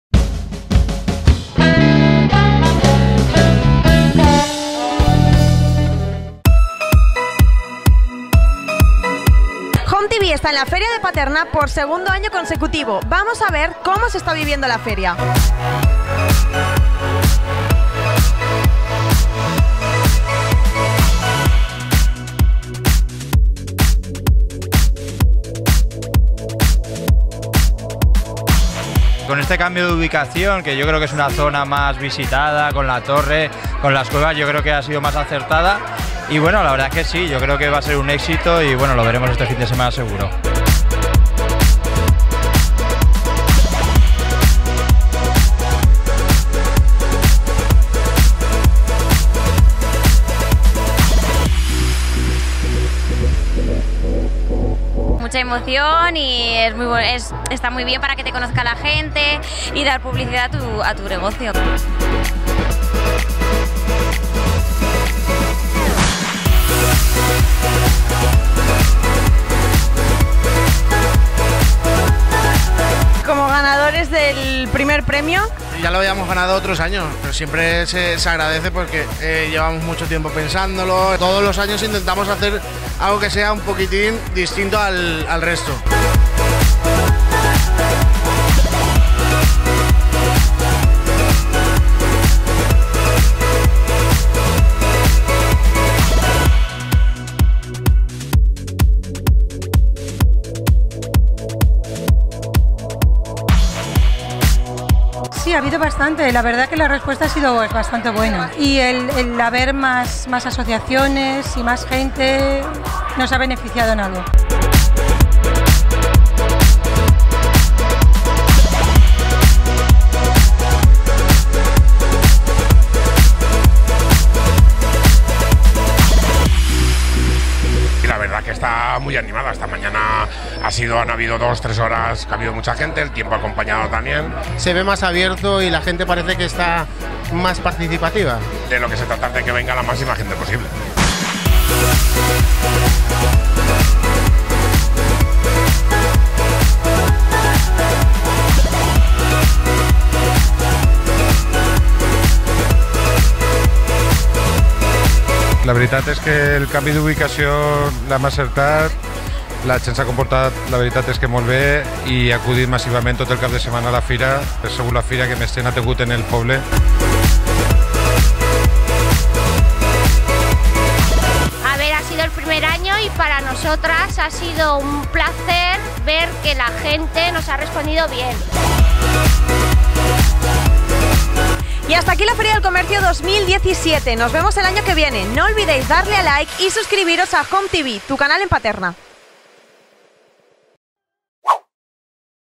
Video: ¡Feria Comercio Paterna 2017 por Home TV!
Home TV estuvo un año más en la Feria comercial, gastronómica y turística de Paterna 2017 y este fue el resultado, comercios contentos y la población de Paterna, ¡todavía más!